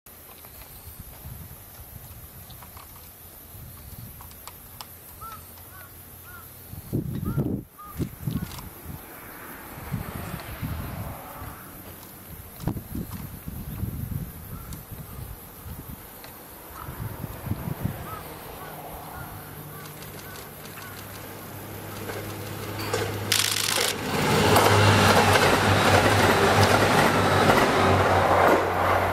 9001D 特急大山1号(2018-08-25)
9001D　特急大山1号
撮影場所：淀江－伯耆大山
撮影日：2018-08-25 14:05